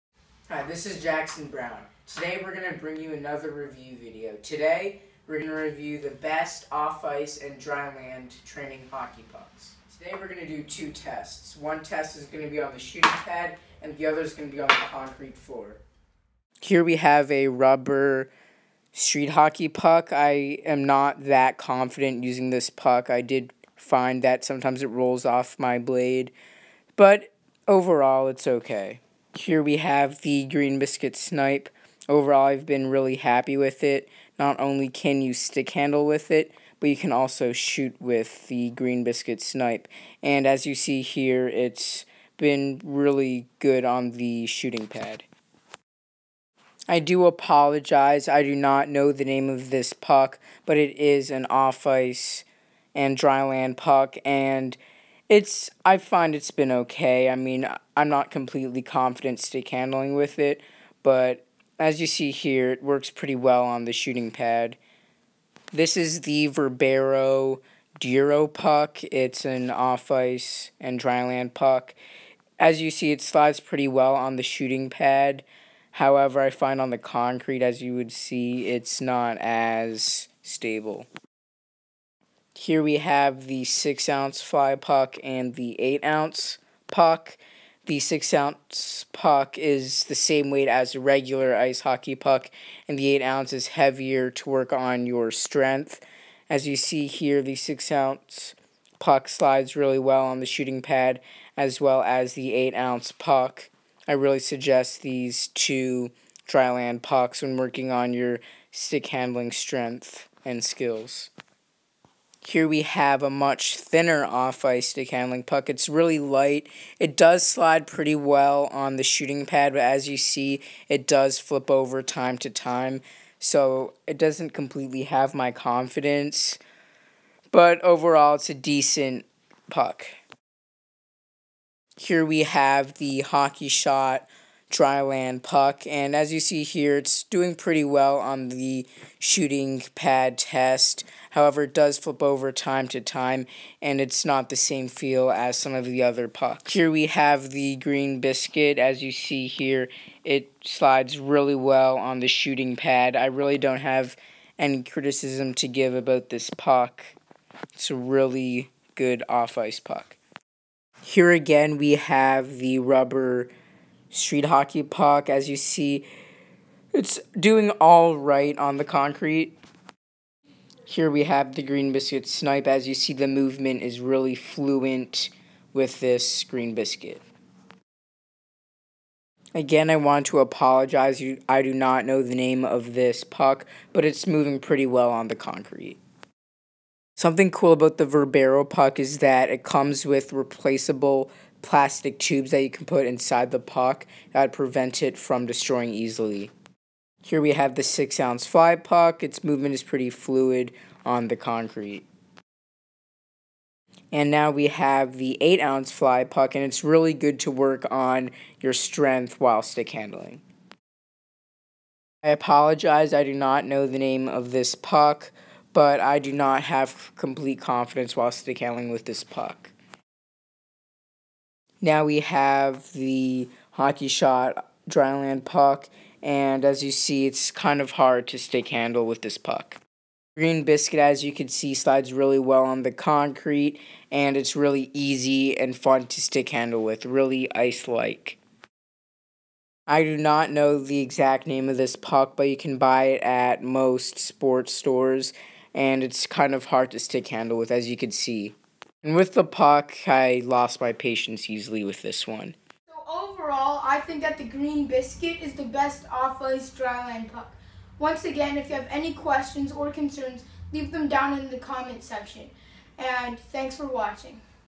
What is the best off-ice hockey puck
To improve your skills in hockey you must practice off-ice. There are many different off-ice hockey pucks. This video will review some of the many off-ice hockey pucks.